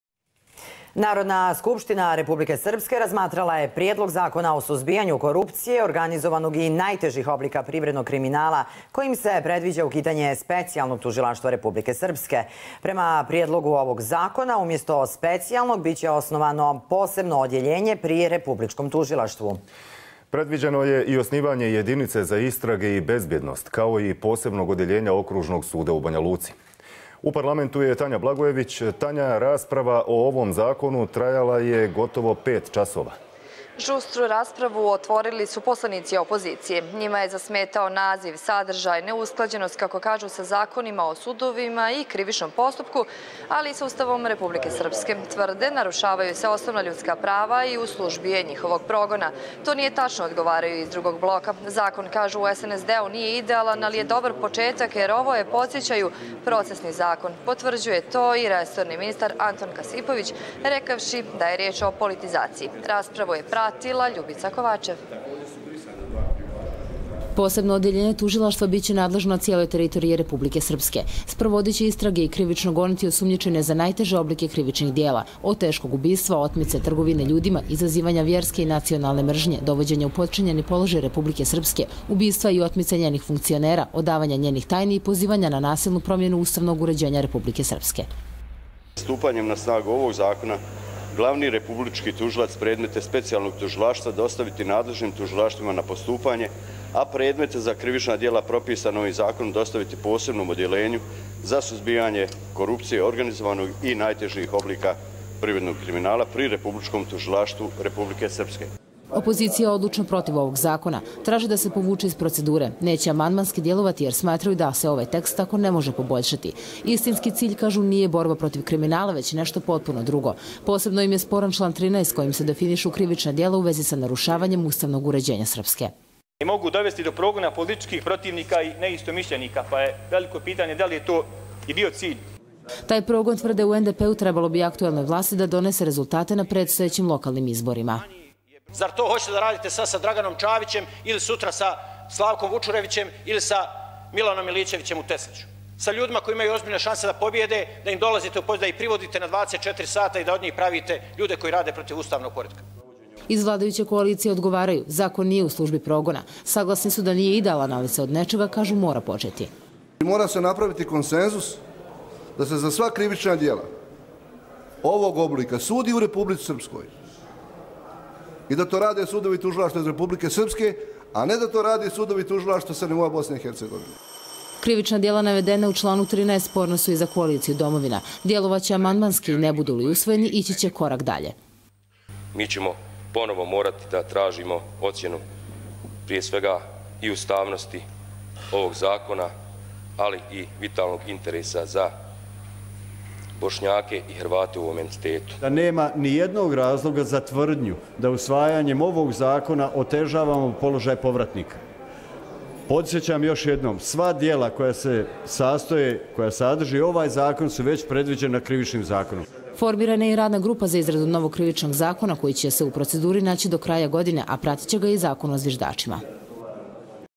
NSRS o Prijedlogu zakona o suzbijanju korupcije
U Banjaluci je u toku deveta redovna sjednica Narodne skupštine Republike Srpske.